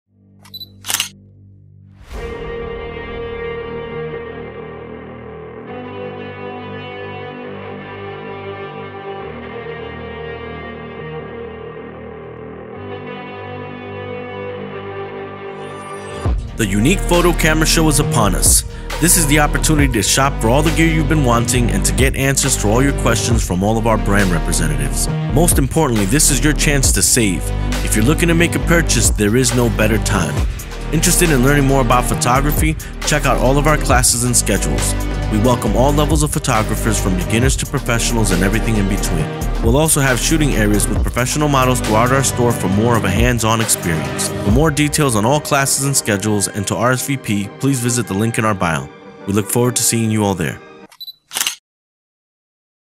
Unique Photo Camera Show 2022 (Fairfield)
It's that time of year! The Unique Photo Camera Show 2022 is upon us and it's going down at both our store locations in New Jersey and Philadelphia.

From Beginners to Professionals... Don't Miss NJ's largest photography event ever.

Learn from world-renowned photographers, interact with manufacturer reps, incredible sales, and much more!

-20+ Brand Vendors
-Crazy Camera & Accessory Pricing
-30+ Free Photography Workshops
-Demo Newest Gear
-Special Performances

For more details on all Classes and Schedules please visit the following